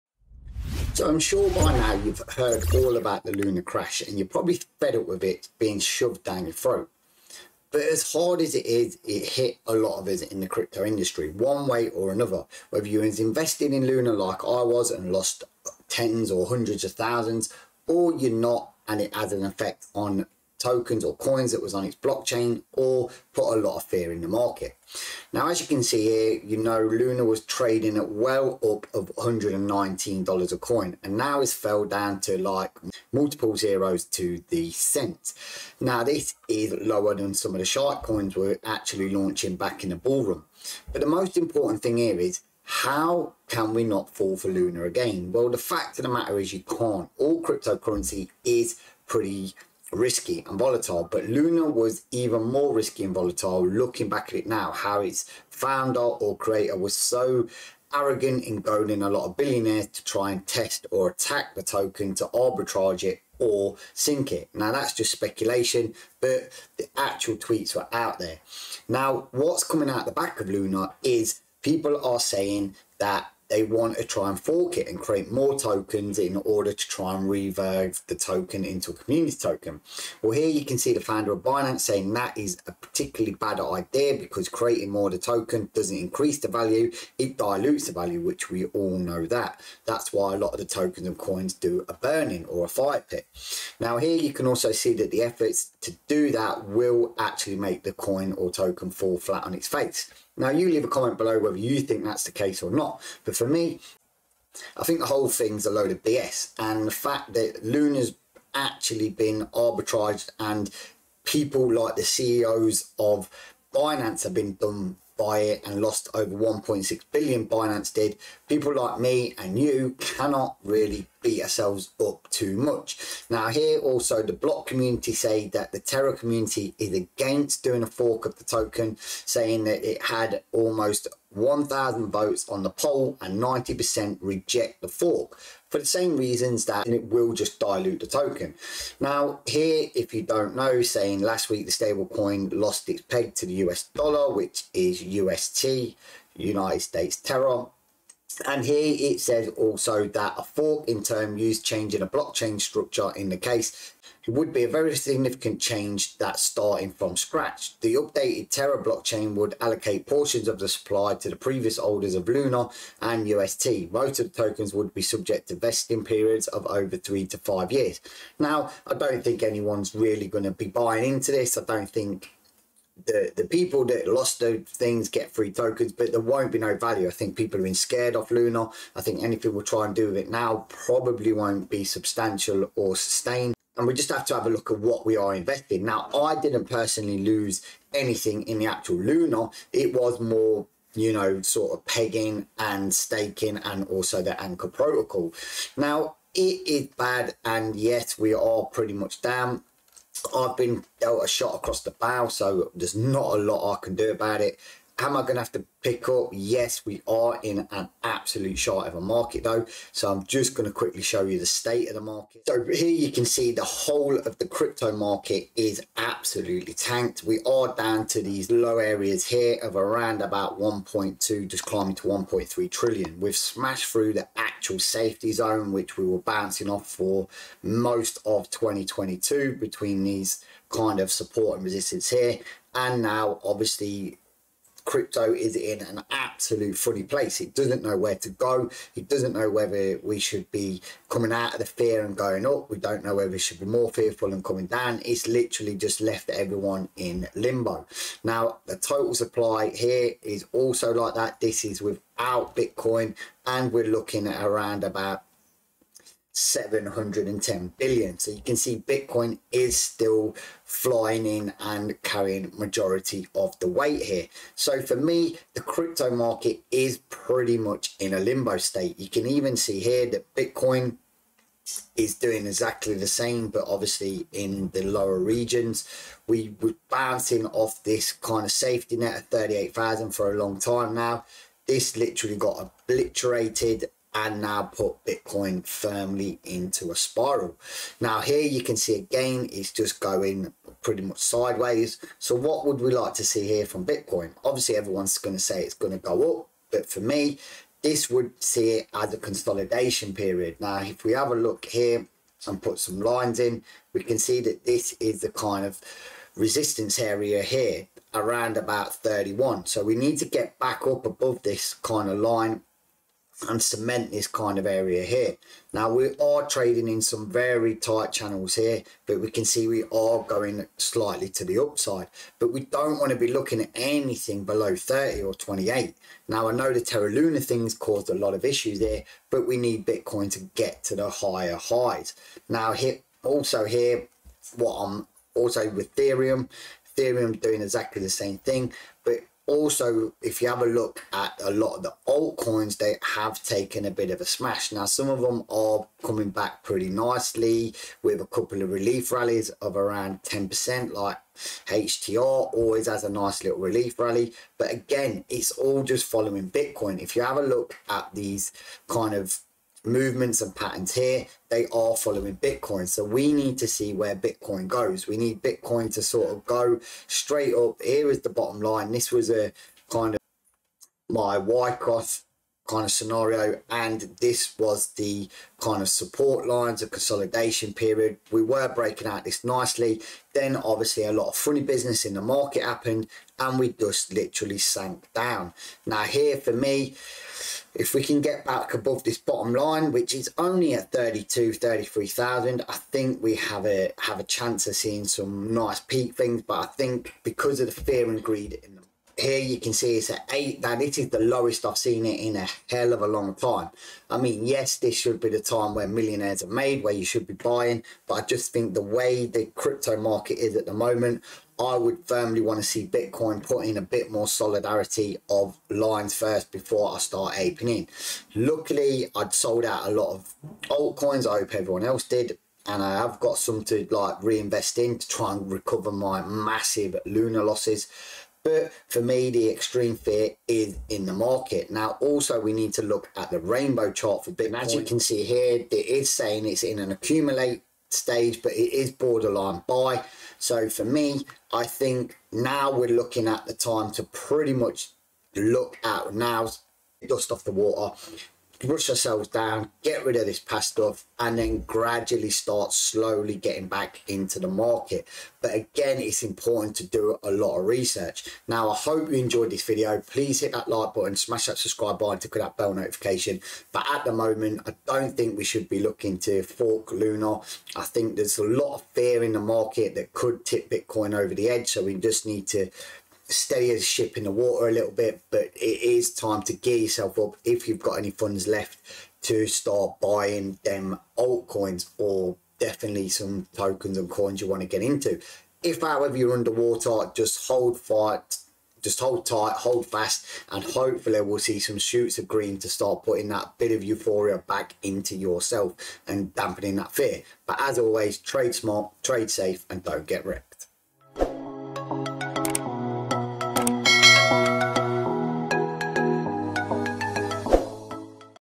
Luna looking to fork! Good Or Bad?
This video is about  Luna looking to fork! Good Or Bad?

If you are feeling GENEROUS
My Public Address to Receive BTC: bc1q52mm2h9akgd05camxgv4dlj79v9ggup3lcx6xj

Time Stamp:
00:00 Intro
0:05  Terra luna news
3:36 Bitcoin chart
7:15 Bitcoin rainbow chart